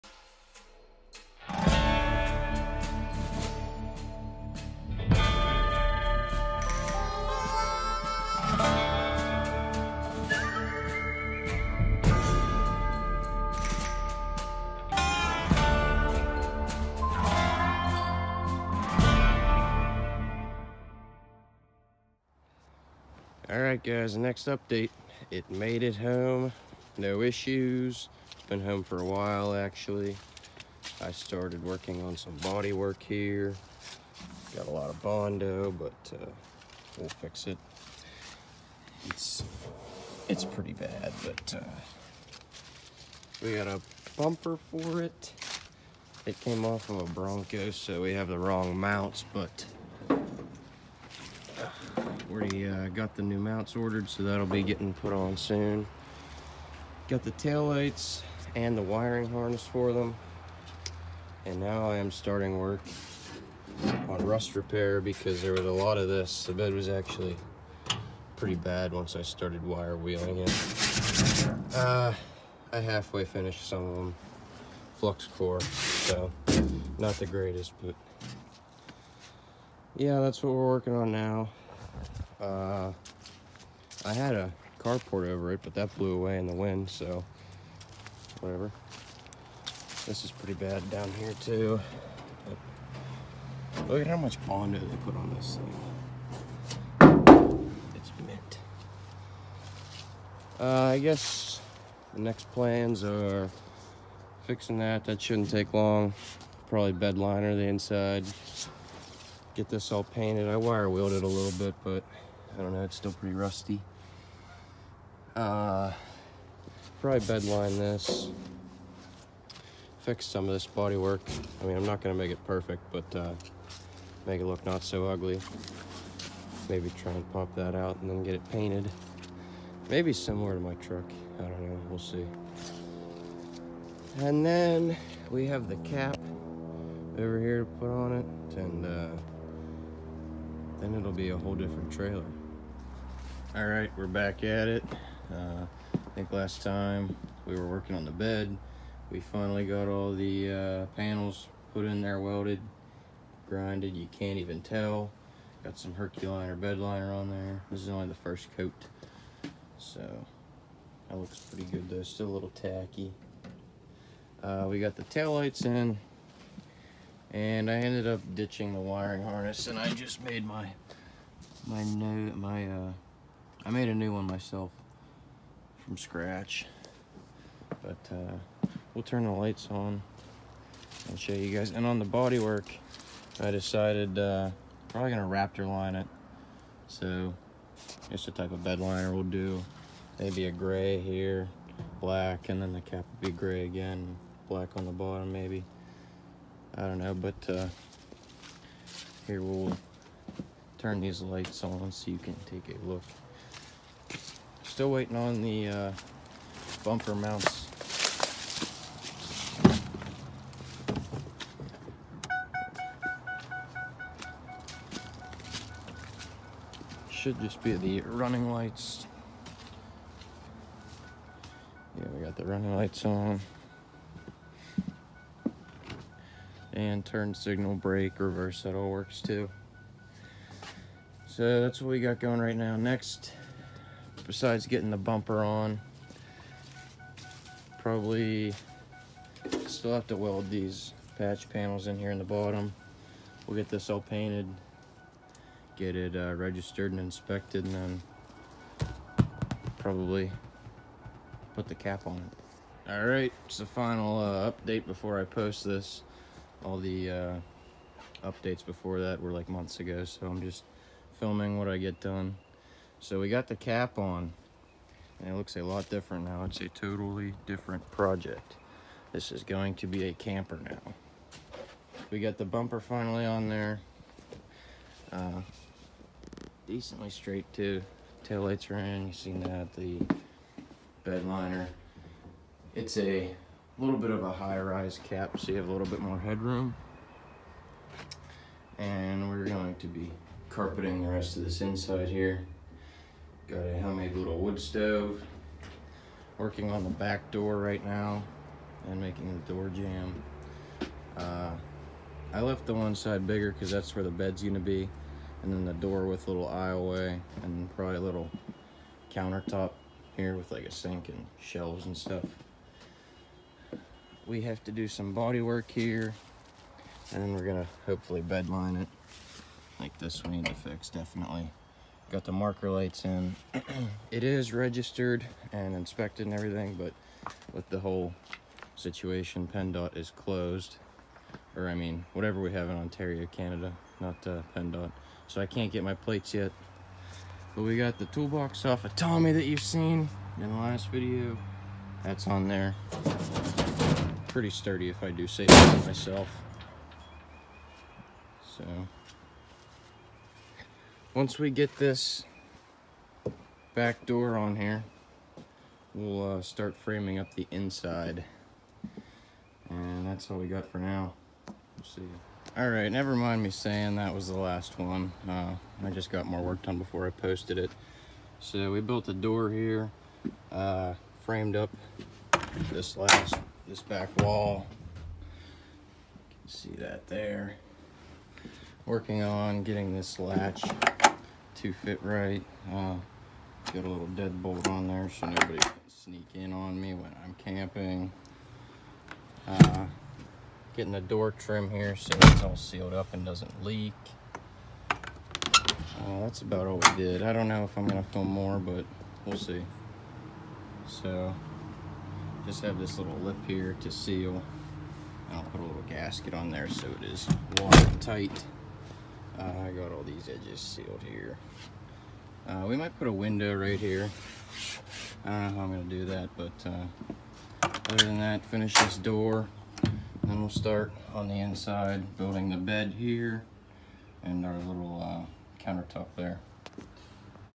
Truck Bed Trailer Camper (pt. 2)
Getting close to finishing the truck bed trailer camper! Let us know what else you want to see on the channel.